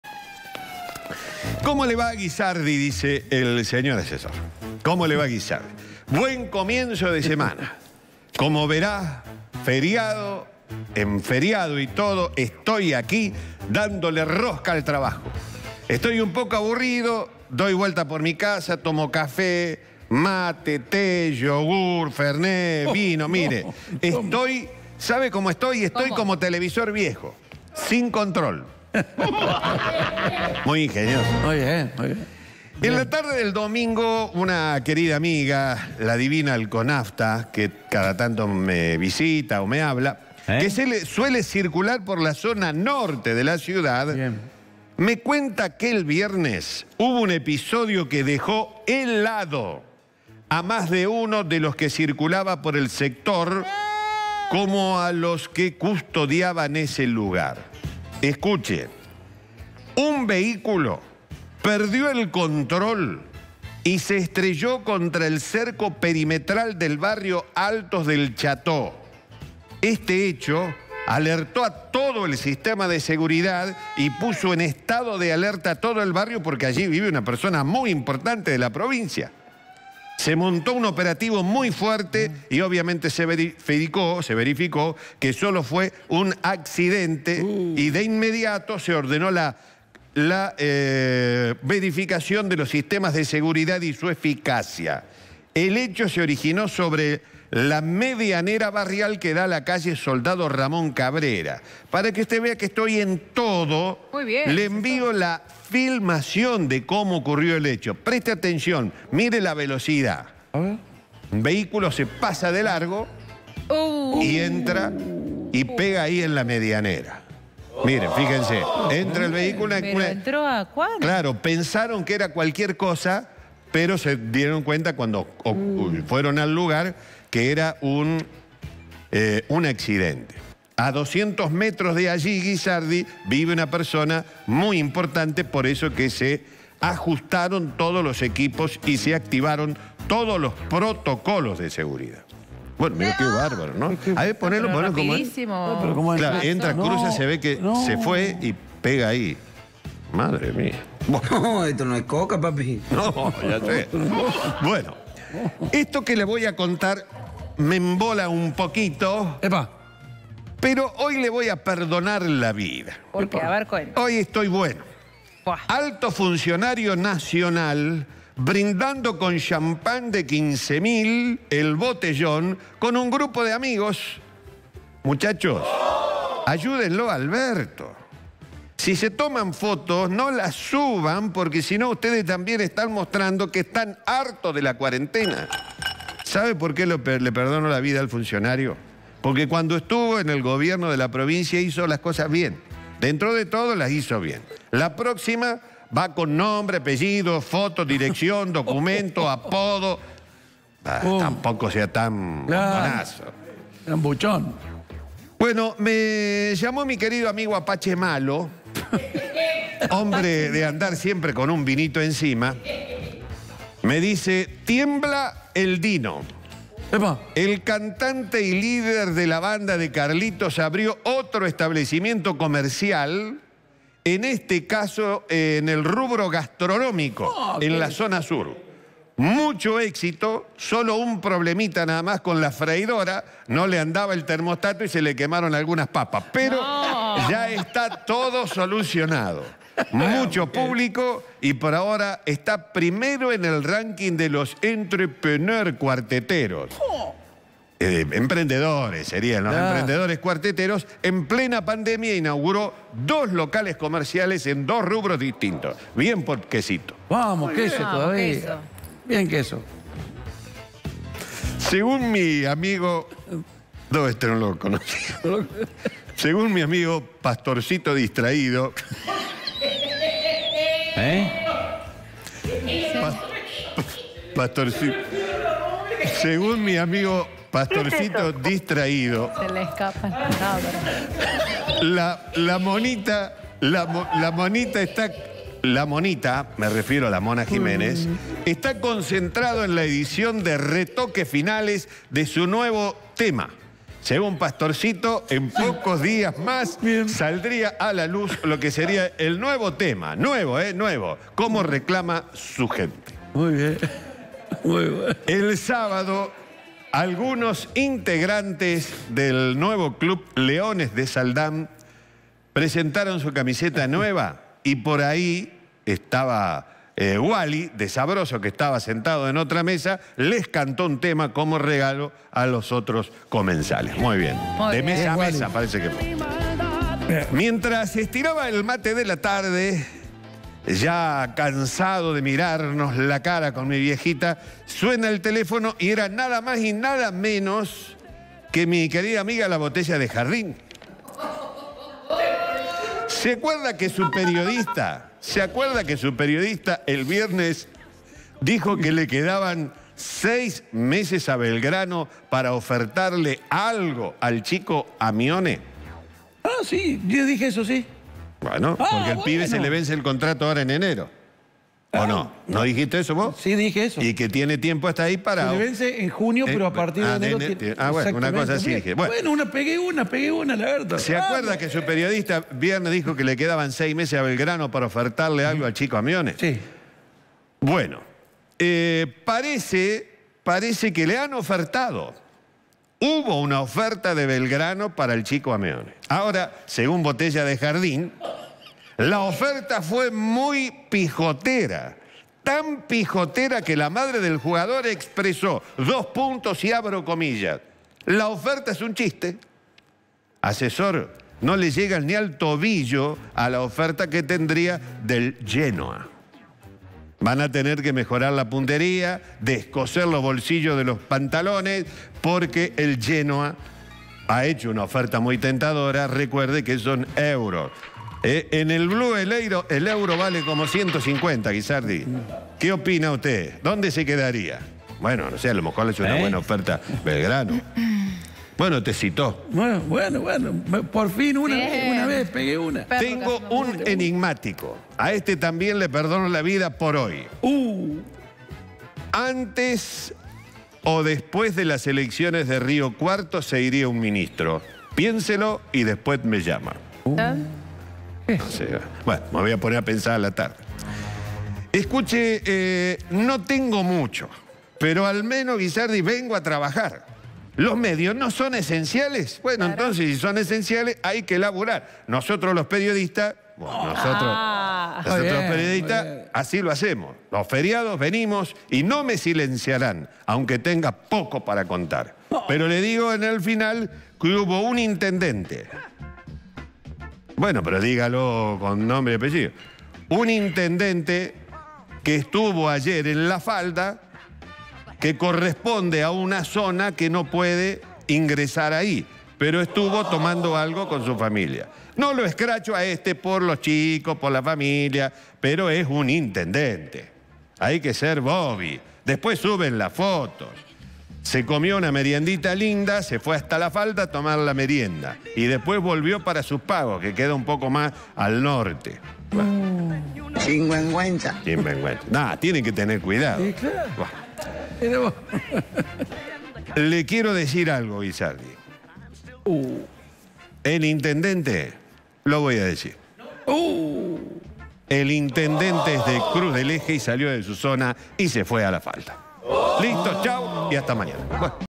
Alerta en un barrio cerrado por incidente
El viernes, ocurrió un episodio que dejó helado a quienes viven en los Altos del Chateau: Un vehículo perdió el control y se estampó contra el cerco perimetral. Debido a la gran velocidad a la que iba, prácticamente destrozó el muro, y obligó al equipo de seguridad a ponerse en estado de alerta, debido a que en ese barrio vive una persona importante. Aún así, el operativo duró poco, luego de que los guardias constataran que fue un accidente.
Entre otras noticias, el Asesor expresó su enojo con un funcionario nacional (que anteriormente estuvo en la Provincia) que se sacó una foto rompiendo la cuarentena y brindando con un champagne de 15.000 pesos. A su vez, "Pastorcito Distraído" comentó que La Mona Jiménez está realizando los últimos retoques a su nuevo material discográfico. Además, "Botella de Jardín" le dijo al Asesor que Belgrano ofertó para mantener a Bruno Amione en el equipo, aunque la oferta fue tan penosa que salió la madre del jugador y dijo que el monto ofertado era «un chiste».
Y a modo de cierre, el intendente de Cruz del Eje, Claudio Sebastián Farías, fue visto pasando a La Falda (que por el momento sigue siendo zona roja), y rompiendo la cuarentena en dicha localidad.